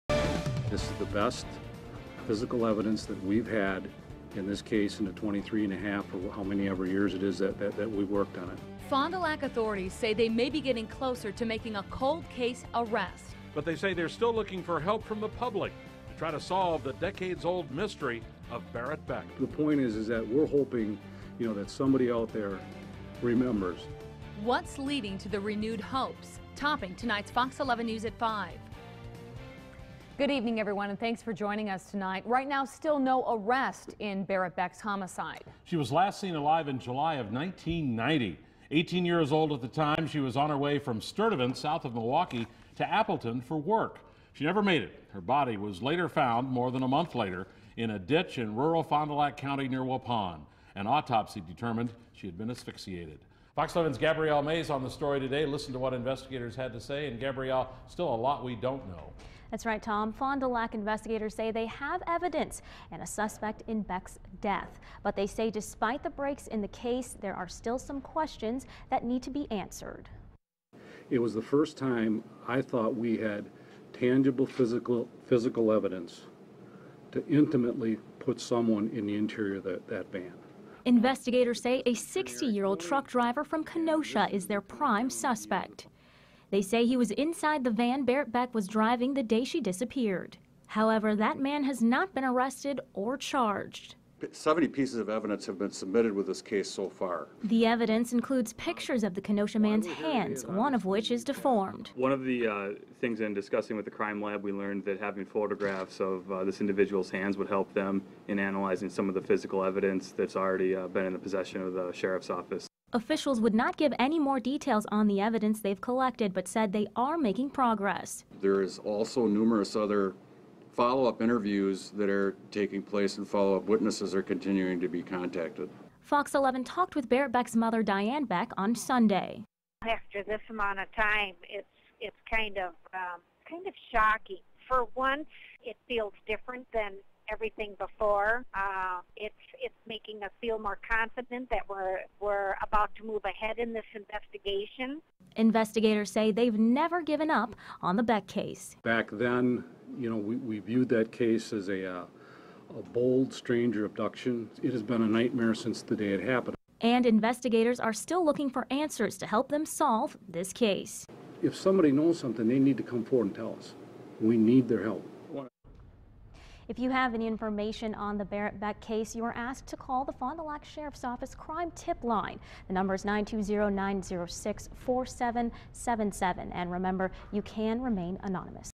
Authorities hope someone remembers something about 1990 homicide
A Kenosha man is the prime suspect in the 1990 slaying of an 18-year-old woman in Fond du Lac County, authorities said Monday.